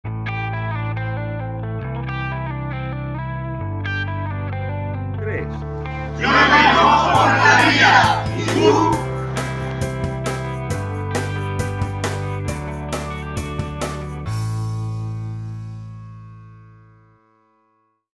La Banda de Estella/Lizarra se moja por la vida, y tú?
Gracias a la banda de música de Estella por mojarse con nosotros, nuestras navarricas cuentan con mucho apoyo.